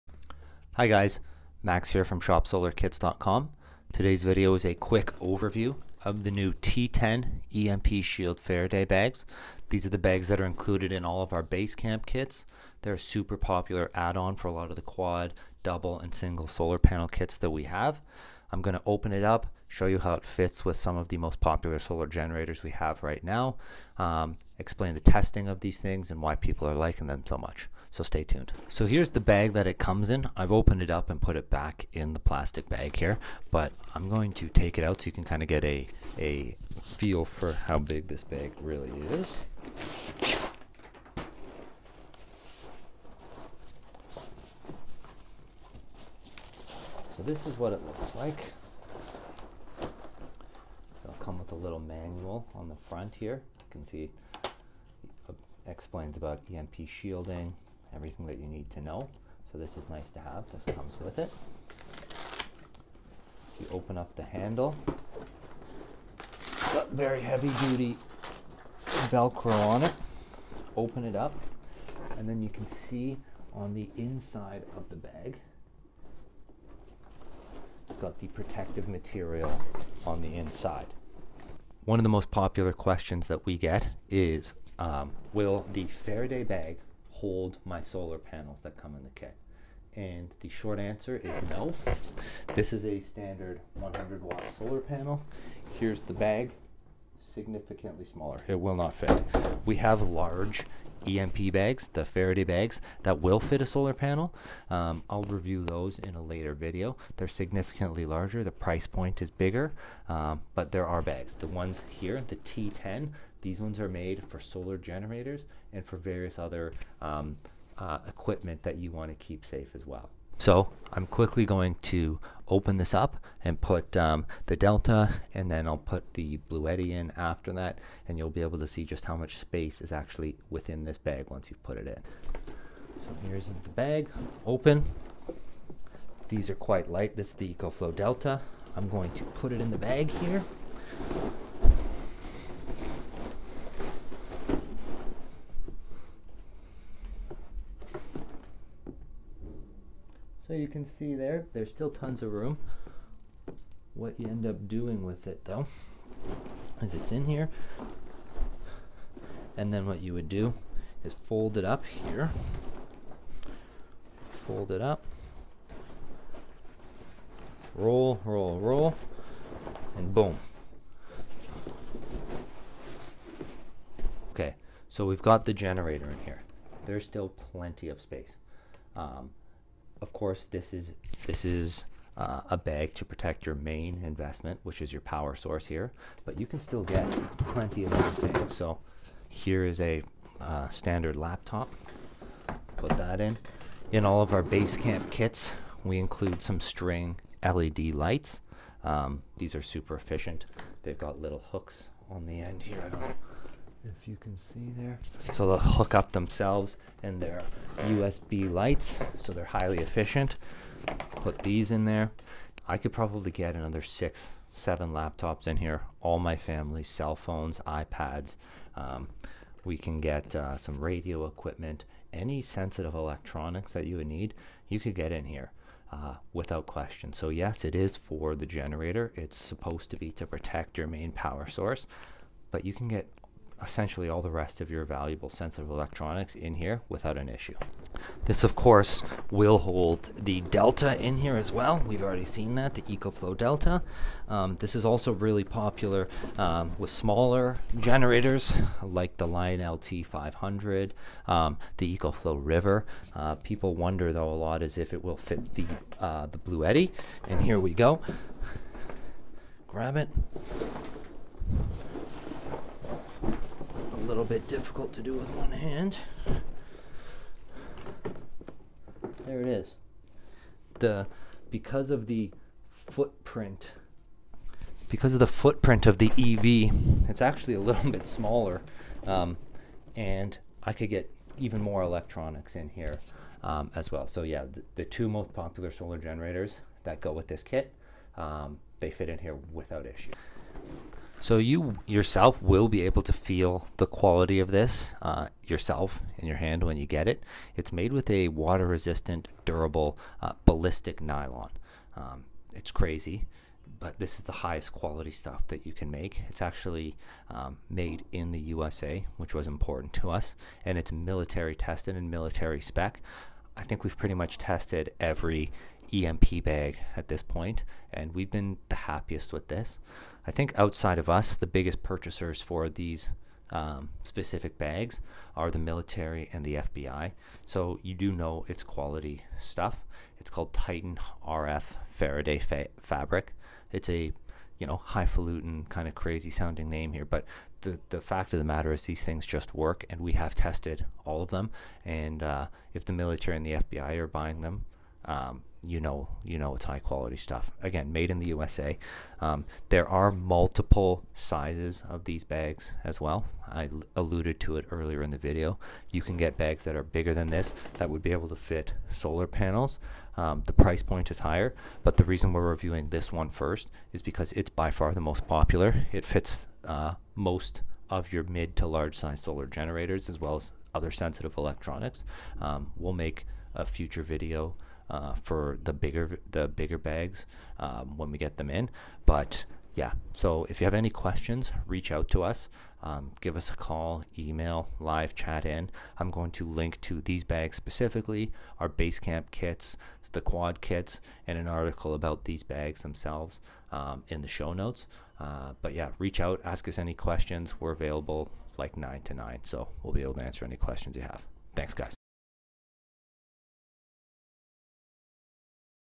EMP Shield / Faraday Bag Review: Best EMP Bag For Portable Solar Generators & Electronics
In this video we review the most popular and highest quality EMP Faraday Bag on the market.  We have tested MANY faraday bags and this is the best one we have been able to fine.  It is made in the USA and used by the military and FBI.